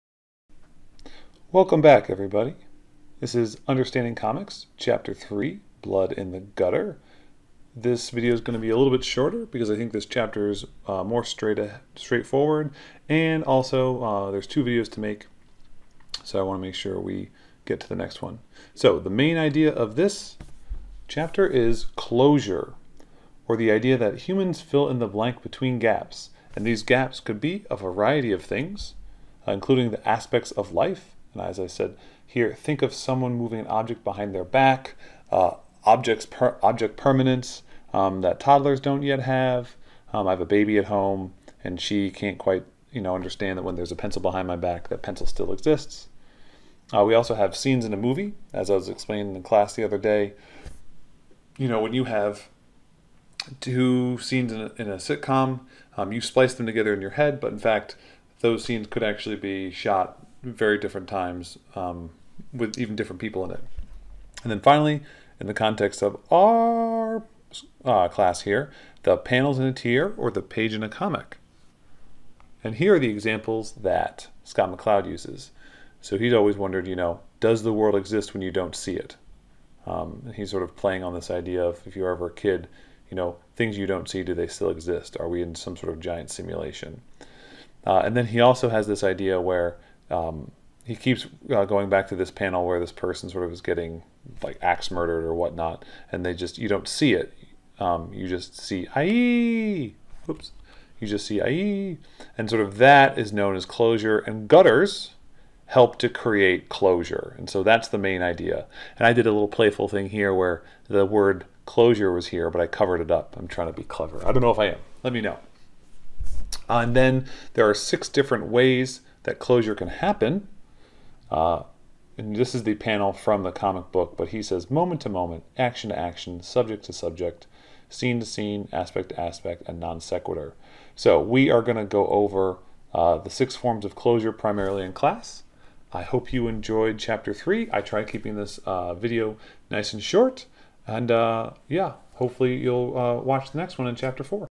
Scott McCloud's Understanding Comics, chapter 3
A short video about Scott McCloud's Understanding Comics, chapter 3 "Blood in the Gutter." McCloud a discusses the concept of closure, which is the idea that humans need to fill in incomplete gaps in knowledge. With respect to comics, closure occurs through the gutter, panel placement, and position of panels/tiers.